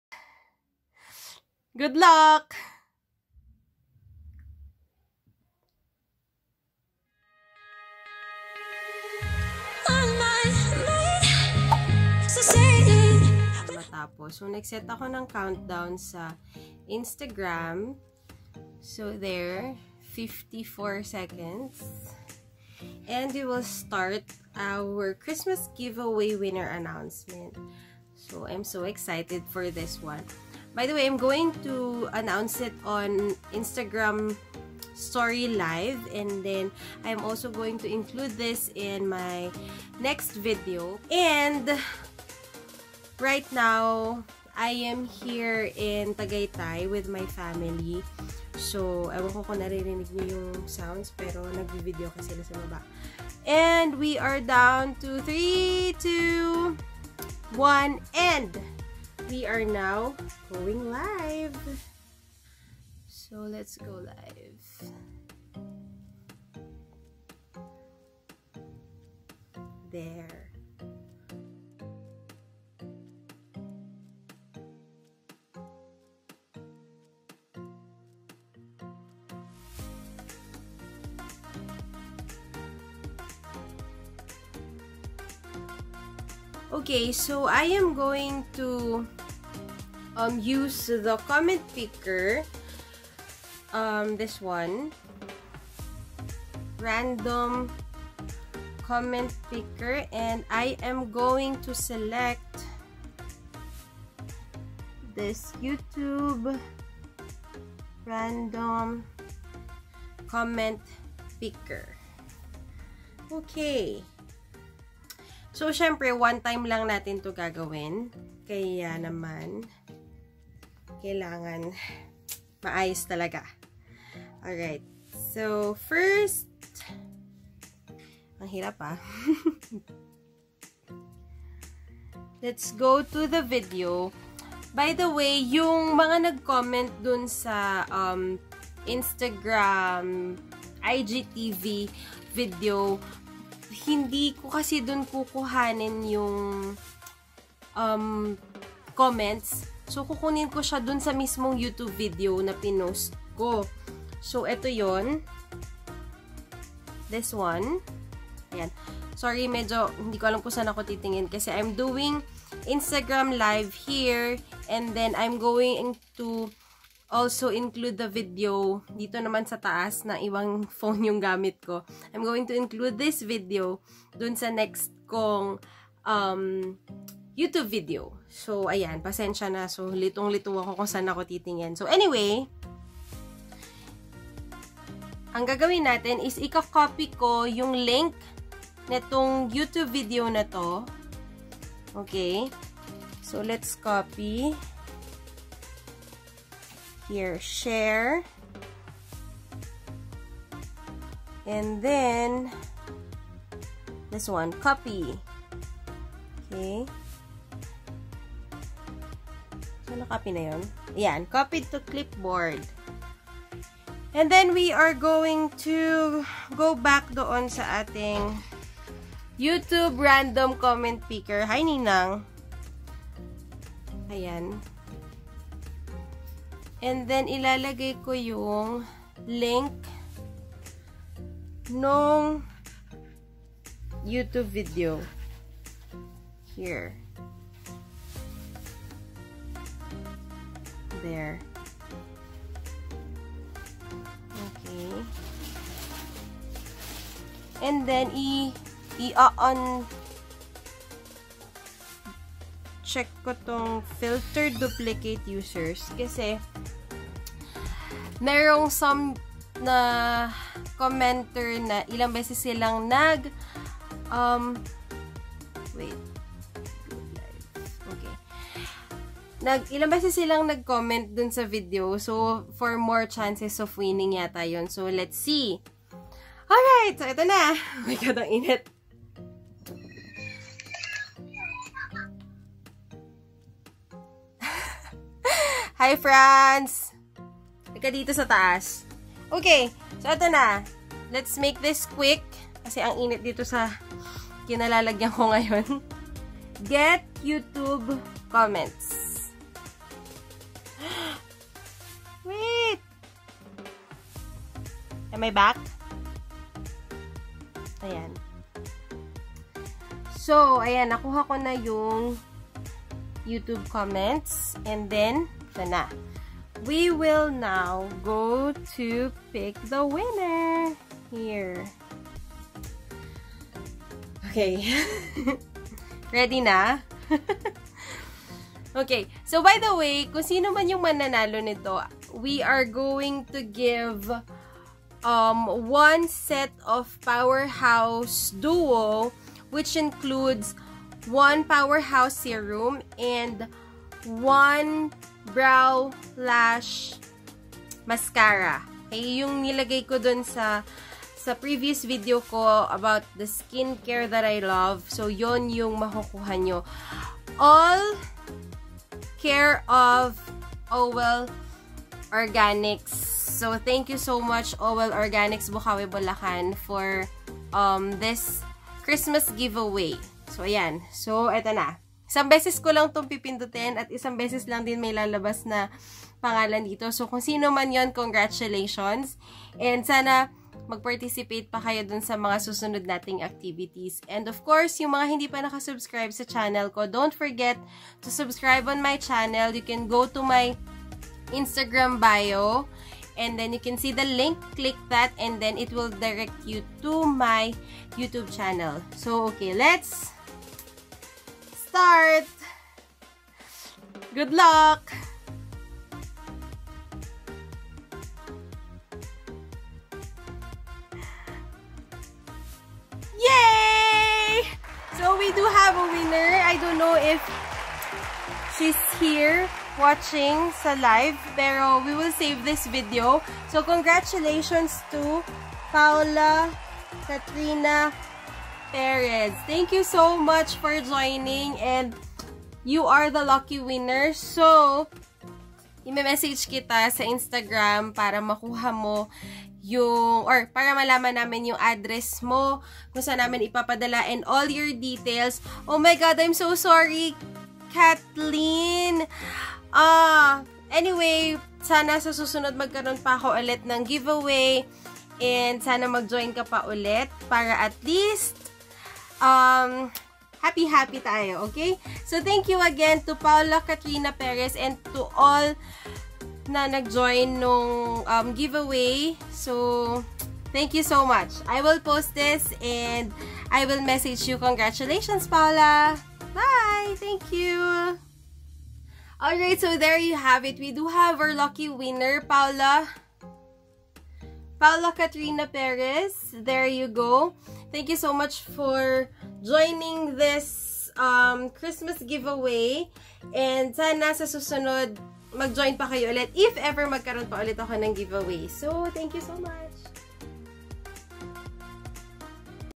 CHRISTMAS GIVEAWAY WINNER ANNOUNCEMENT | Gifts from me and Oh Well Bocaue Bulacan! ✨
Congratulations to our winner and thanks to everyone who joined! ❤️✨